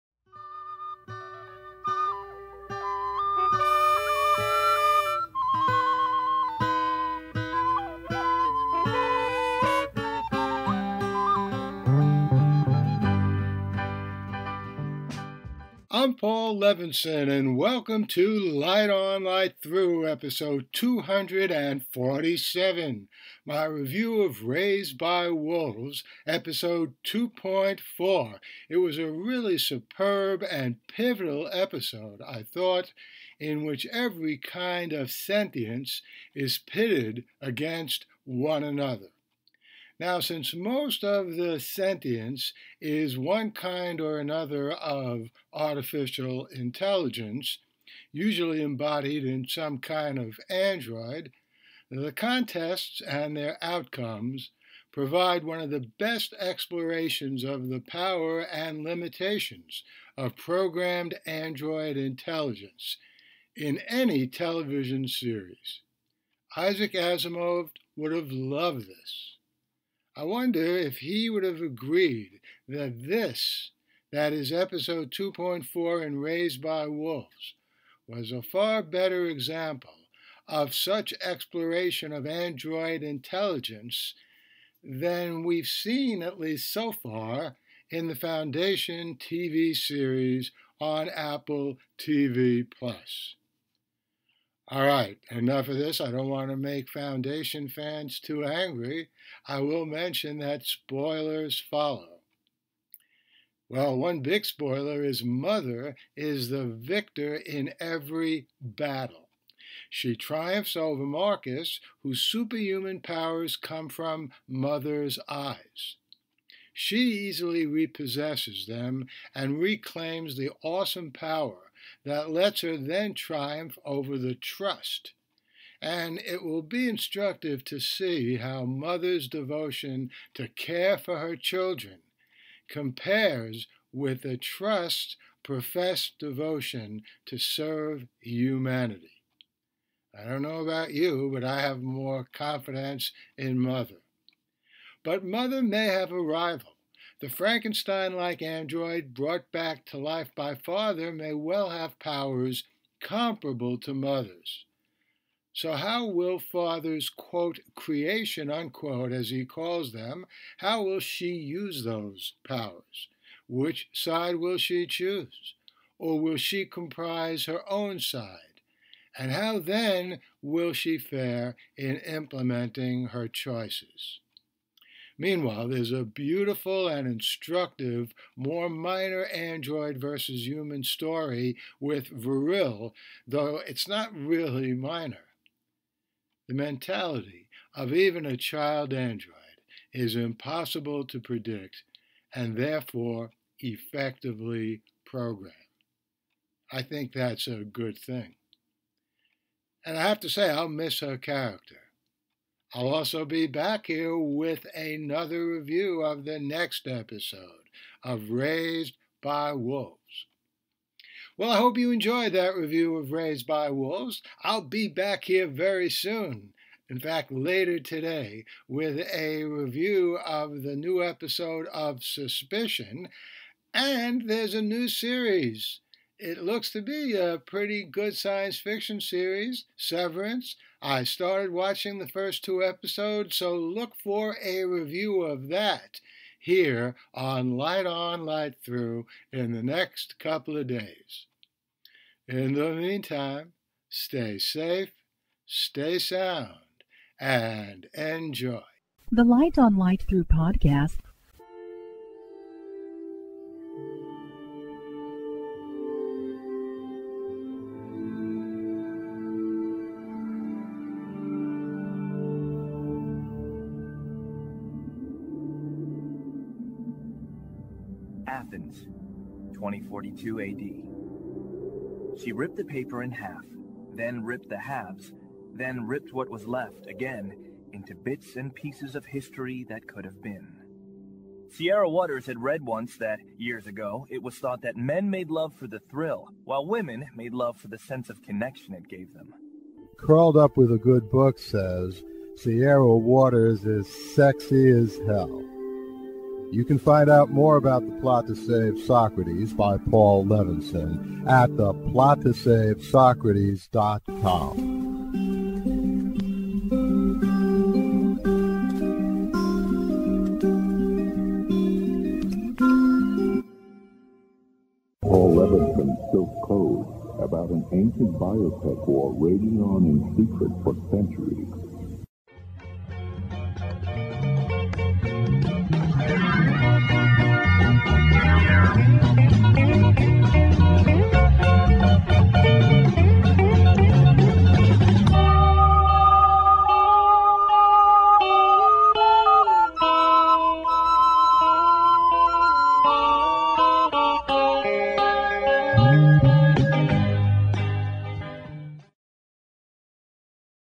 Review of Raised by Wolves 2.4
Welcome to Light On Light Through, Episode 247, in which I review Raised by Wolves episode 2.4 on HBO Max.

Further reading:

Touching the Face of the Cosmos: On the Interaction of Space and Religion